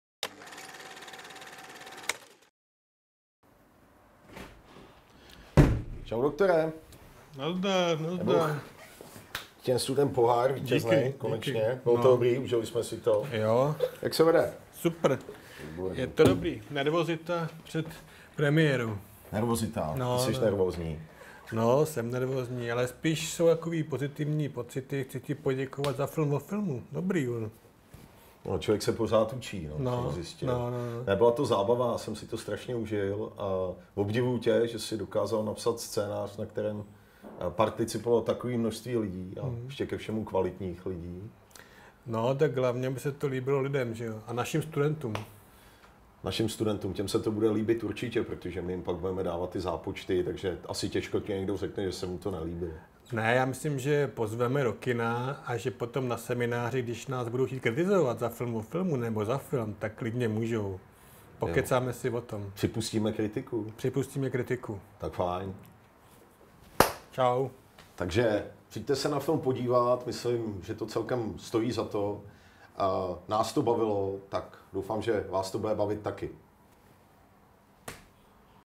Pozvánka na film Zlatý podraz
Jiří Závozda a Roman Bradáč zvou diváky na nový celovečerní film, na kterém se podíleli pedagogové a studenti MUP. Roman Bradáč pak natočil se studenty Film o filmu, který několikrát v říjnu odvysílá ČT